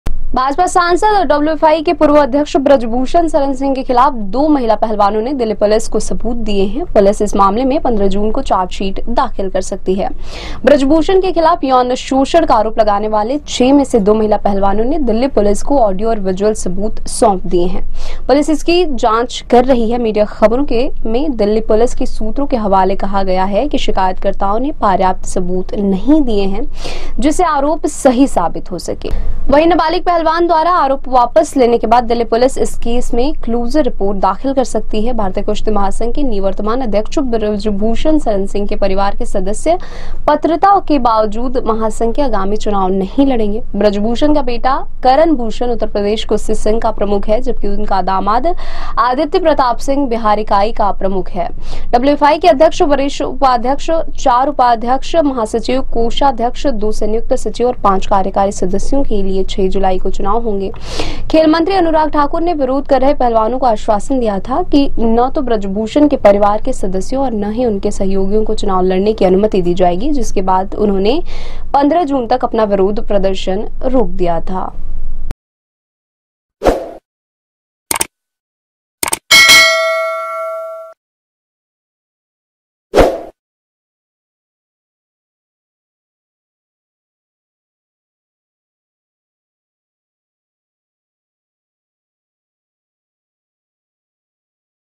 Wrestlers Protest: बृजभूषण मुश्किल में, 2 महिला पहलवानों ने Delhi पुलिस को ऑडियो और विजुअल सबूत दिए
बृजभूषण के खिलाफ यौन शोषण का आरोप लगाने वाली 6 में से 2 महिला पहलवानों ने दिल्ली पुलिस को ऑडियो और विजुअल सबूत सौंप दिए हैं। पुलिस इनकी जांच कर रही है। मीडिया खबरों में दिल्ली पुलिस के सूत्रों के हवाले से कहा गया है कि शिकायतकर्ताओं ने पर्याप्त सबूत नहीं दिए हैं जिससे आरोप सही साबित हो सके। वहीं, नाबालिग पहलवान द्वारा आरोप वापस लेने के बाद पुलिस इस केस में क्लोजर रिपोर्ट दाखिल कर सकती है।

भारतीय कुश्ती महासंघ के निवर्तमान अध्यक्ष बृजभूषण शरण सिंह के परिवार के सदस्य पात्रता के बावजूद महासंघ के आगामी चुनाव नहीं लड़ेंगे। बृजभूषण का बेटा करण भूषण उत्तर प्रदेश कुश्ती संघ का प्रमुख है जबकि उसका दामाद आदित्य प्रताप सिंह बिहार इकाई का प्रमुख है। WFI के अध्यक्ष, वरिष्ठ उपाध्यक्ष, चार उपाध्यक्ष, महासचिव, कोषाध्यक्ष, दो संयुक्त सचिव और पांच कार्यकारी सदस्यों के लिए 6 जुलाई को चुनाव होंगे।